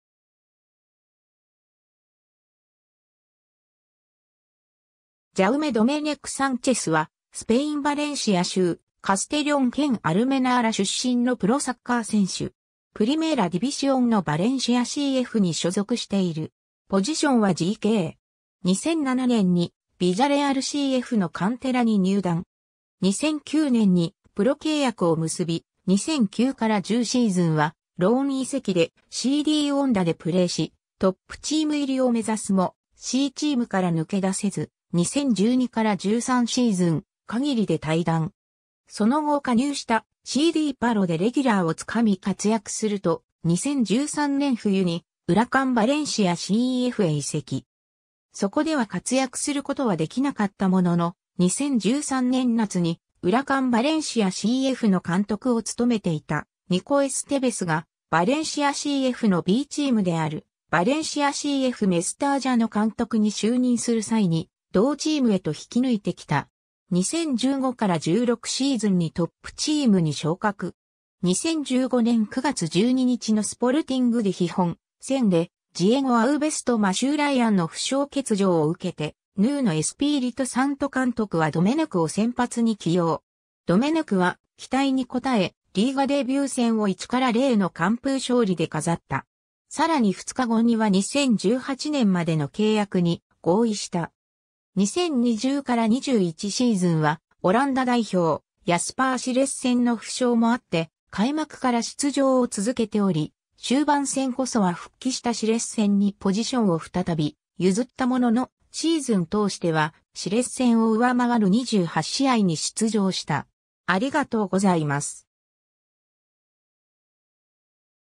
ジャウメ・ドメネク
バレンシアCF・メスタージャの選手
カステリョー県/カステリョン県出身の人物
ジャウメ・ドメネク・サンチェス（Jaume Domènech Sánchez , 1990年11月5日 - ）は、スペイン・バレンシア州カステリョン県アルメナーラ出身のプロサッカー選手。
プリメーラ・ディビシオンのバレンシアCFに所属している。
 2007年にビジャレアルCFのカンテラに入団。
2009年にプロ契約を結び、2009-10シーズンはローン移籍でCDオンダでプレーし、トップチーム入りを目指すも、Cチームから抜け出せず、2012-13シーズン限りで退団。
 その後加入したCDパロでレギュラーを掴み活躍すると、2013年冬にウラカン・バレンシアCFへ移籍。
そこでは活躍することはできなかったものの、2013年夏にウラカン・バレンシアCFの監督を務めていたニコ・エステベスがバレンシアCFのBチームであるバレンシアCF・メスタージャの監督に就任する際に同チームへと引き抜いてきた。
2015-16シーズンにトップチームに昇格。
2015年9月12日のスポルティング・デ・ヒホン戦で、ジエゴ・アウヴェスとマシュー・ライアンの負傷欠場を受けて、ヌーノ・エスピーリト・サント監督はドメネクを先発に起用。
ドメネクは期待に応え、リーガデビュー戦を1-0の完封勝利で飾った。
更に2日後には2018年までの契約に合意した。
 2020-21シーズンは、オランダ代表ヤスパー・シレッセンの負傷もあって開幕から出場を続けており、終盤戦こそは復帰したシレッセンにポジションを再び譲ったものの、シーズン通してはシレッセンを上回る28試合に出場した。